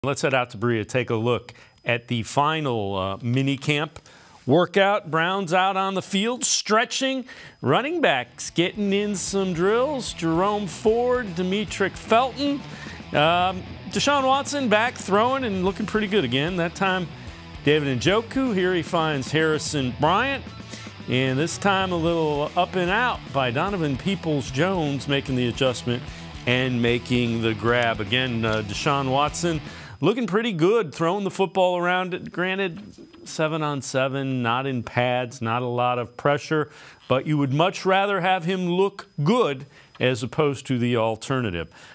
Biggest Highlights From the Final Day of Browns Mandatory Minicamp - Sports4CLE, 6/8/23
Dave Bacon breaks down some of the action from the final day of Browns mandatory minicamp.